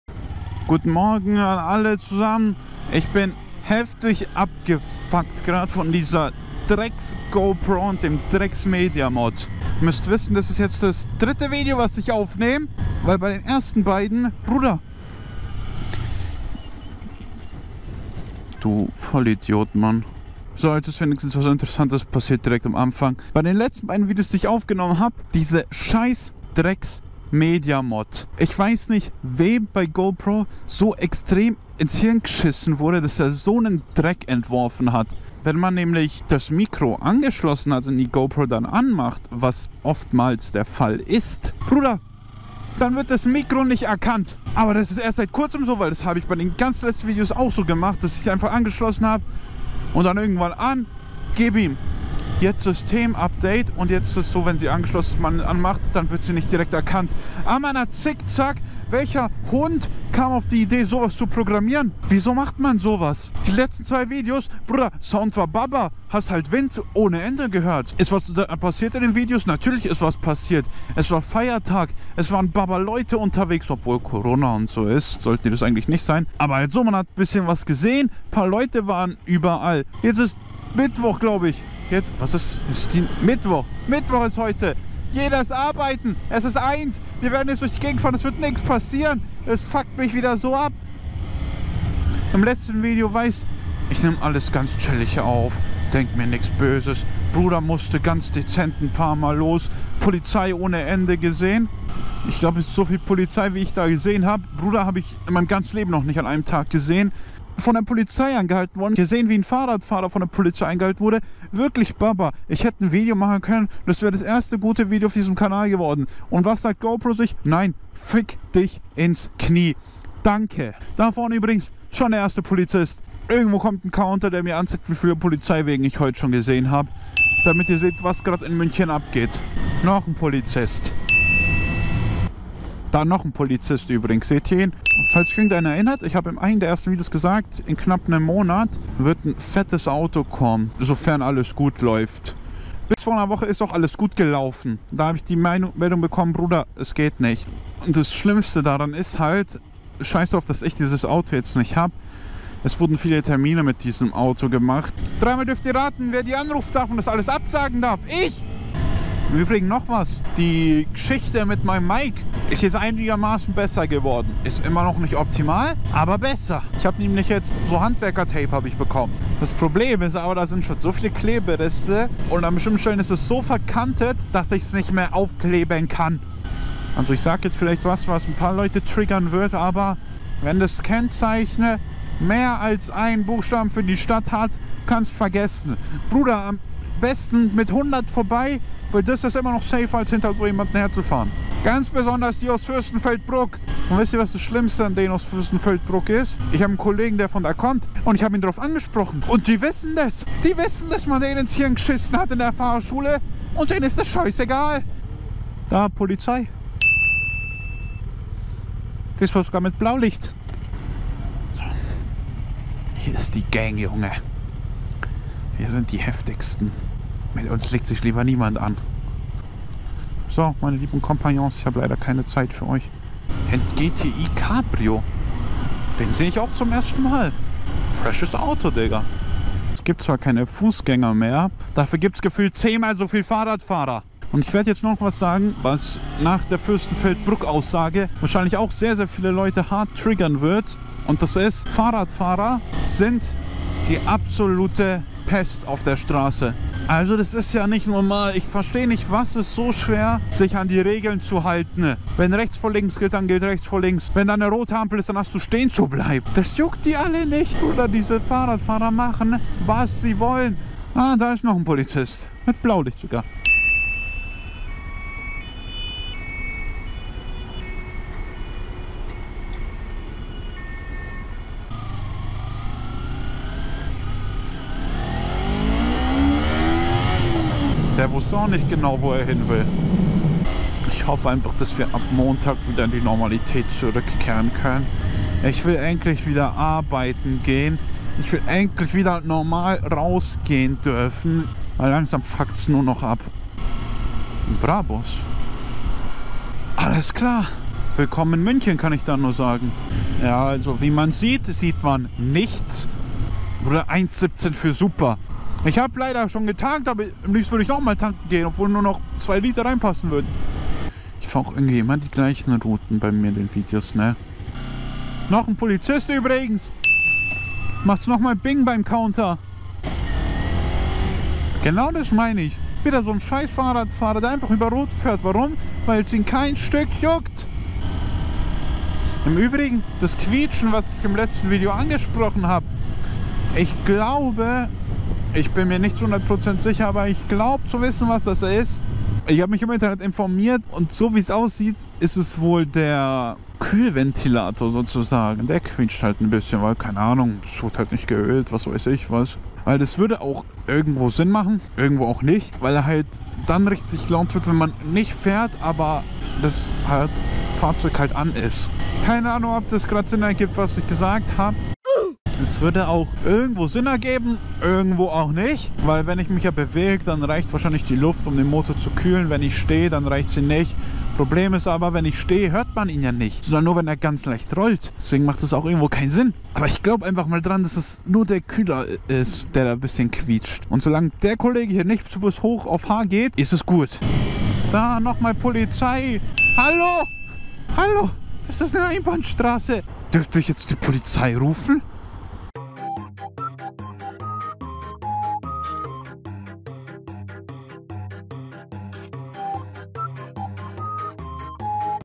Polizei bricht das Gesetz! | Nicht_Dani
Heute ein kleiner Ausraster am Anfang zu dem ach so tollen Media Mod von der GoPro Hero 8 nachdem wir fast nen Unfall haben. Am Ende sehen wir sogar wie die Polizei schwerkriminell das Gesetz bricht.